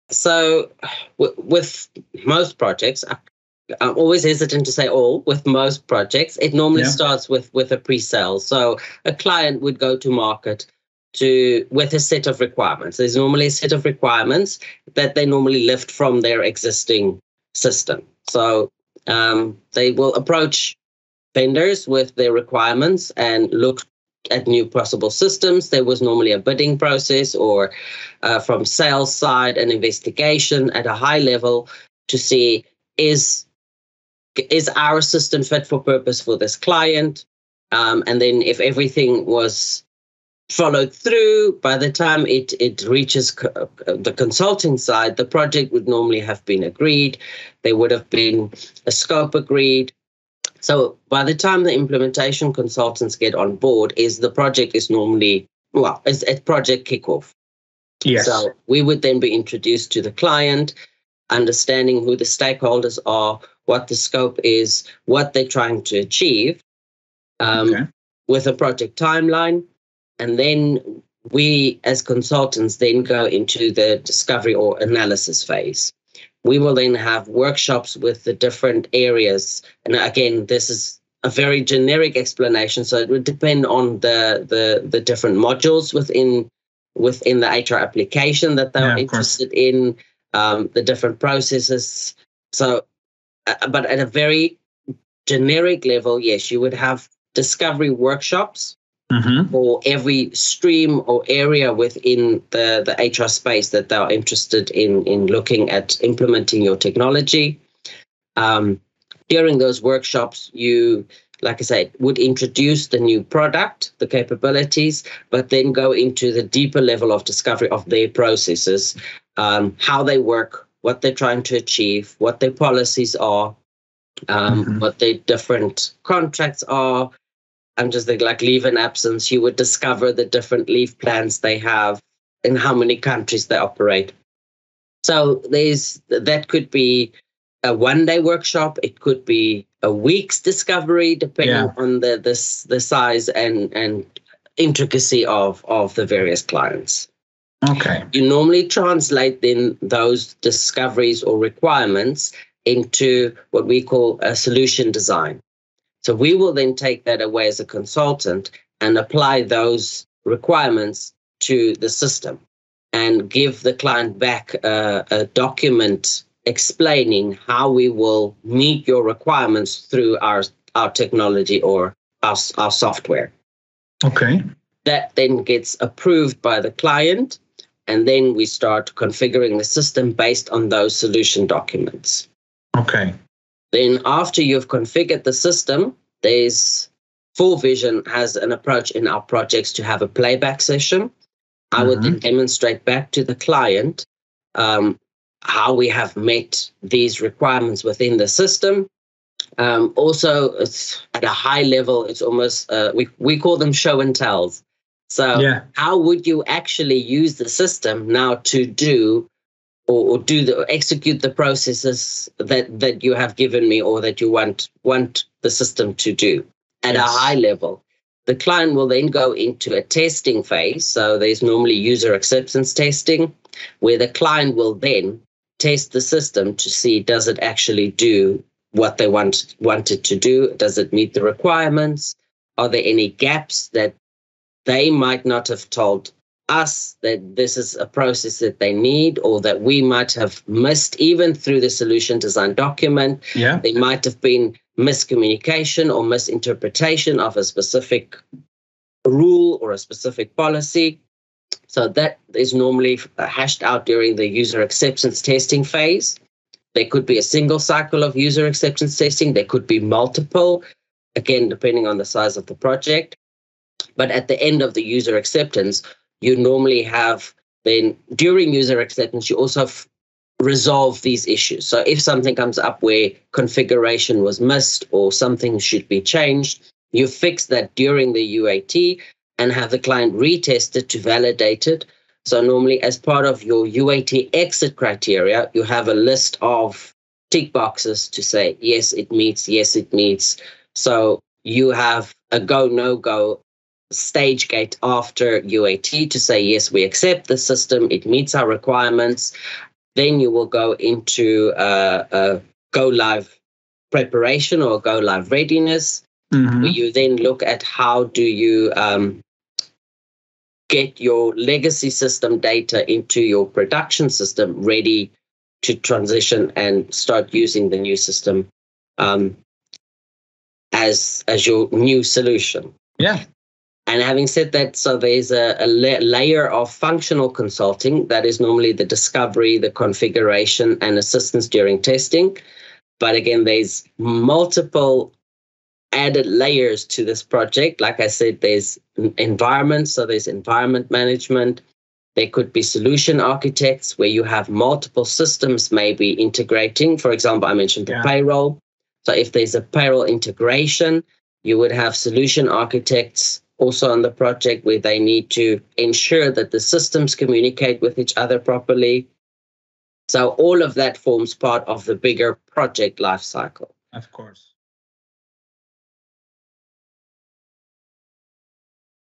HR tech implementation in a nutshell
Here’s the step-by-step HR technology implementation process explained by Stephan Schoeman.

Published by FourVision. Feel free to reuse this work anywhere with attribution to FourVision or the HR Vision Podcast.